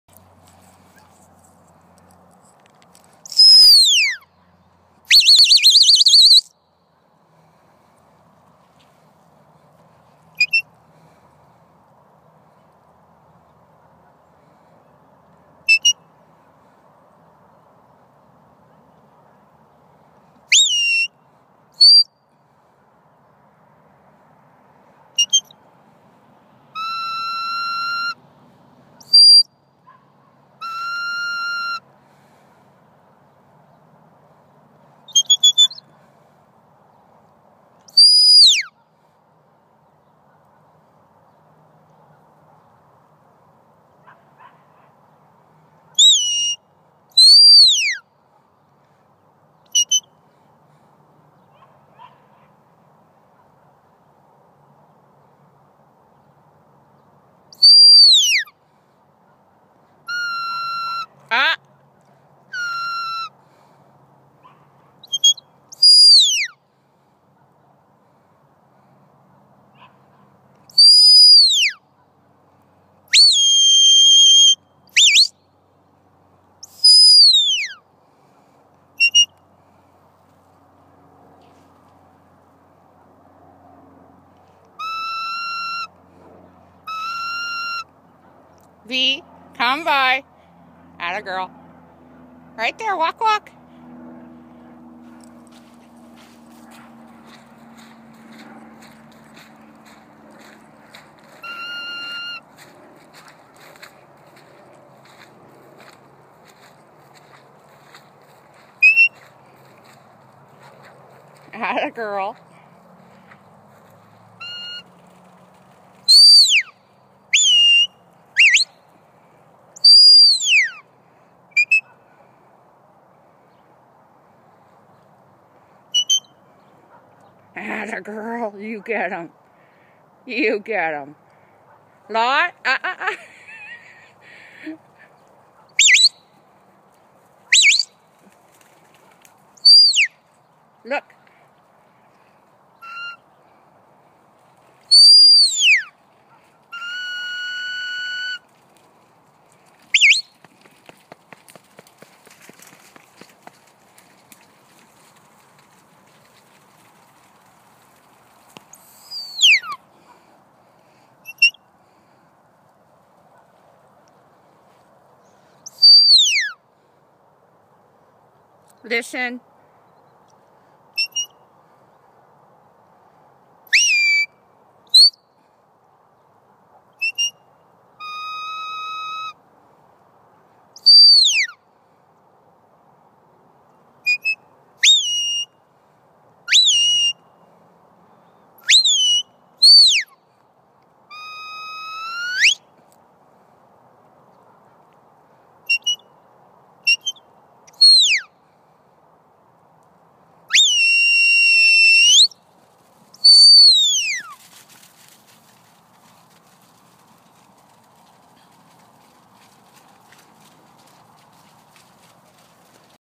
Bea 2 - 2 Jan 14
Bea working naughty puppy sheep. I am purposely putting them in the hardest spots of our pasture so she can think and feel. Not really worried about things being pretty - just want her to work past the pressure.